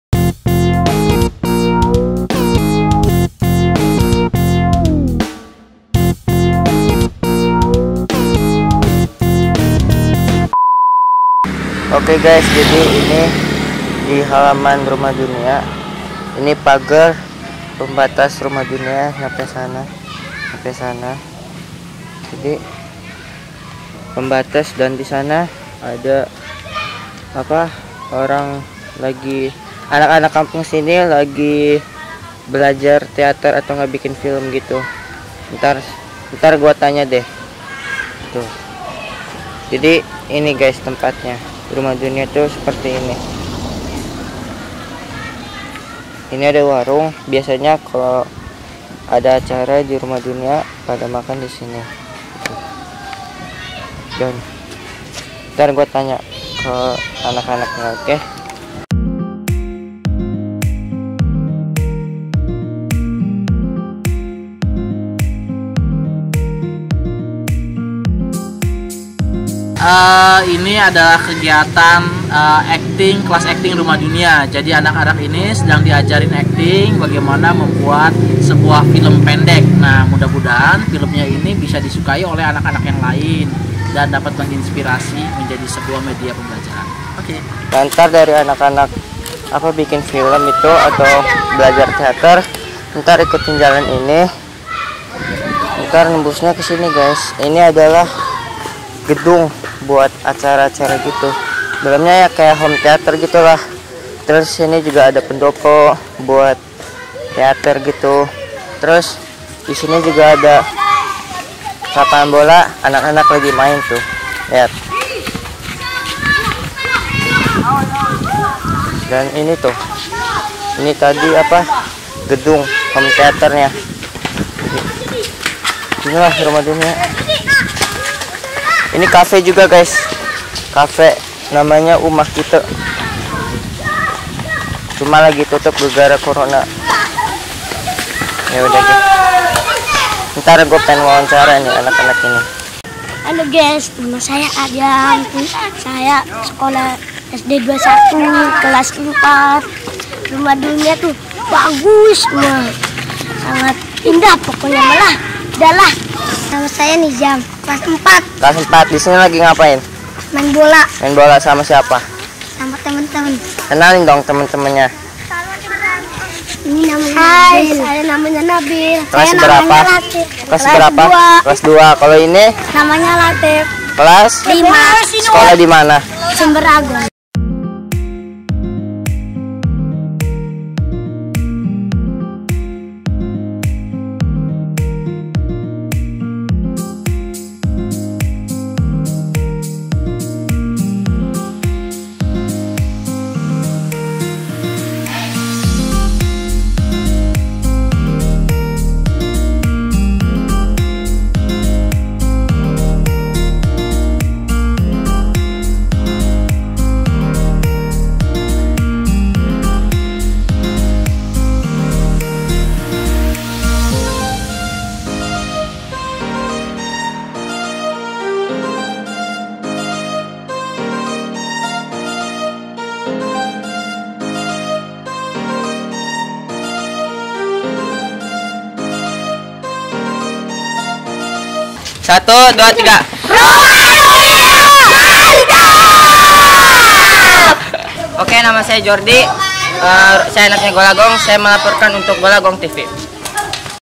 [KOTA SERANG] Rumah Dunia Kawasan Aman Untuk Anak
Jordy Al Ghifari, putra ketiga, mulai beljar bikin vlog. Dia melaporkan kegiatan di Rumah Dunia.

Contact business :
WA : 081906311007